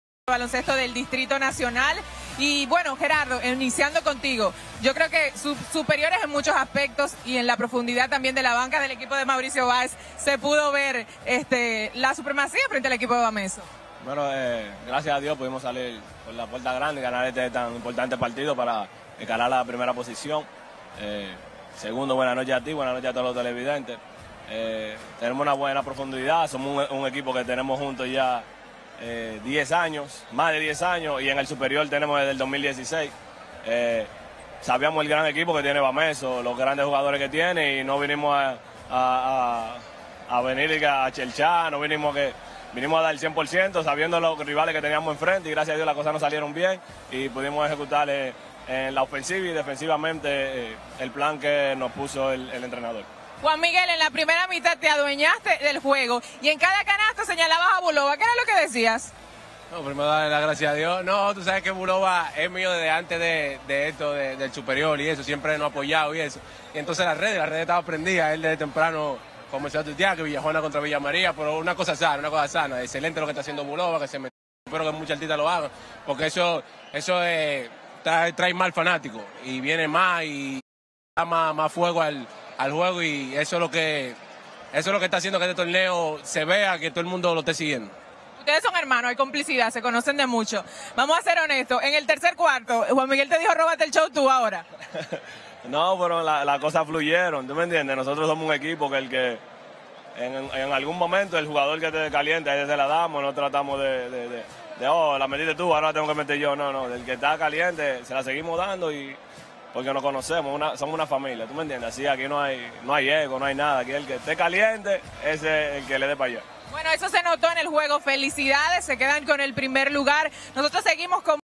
Entrevista a Los Hermanos Suero Geraldo y Juan Miguel 🔥🔥🔥 Logran poner a Mauricio en 1er Lugar TBS-D
Entrevista con Los hermanos Suero
Geraldo y Juan Miguel 🔥🔥🔥

Logran poner al Equipo de Mauricio Báez en el 1er Lugar Derrotando al Bameso  TBS del Distrito 2022

97 Mauricio Báez
78 Bameso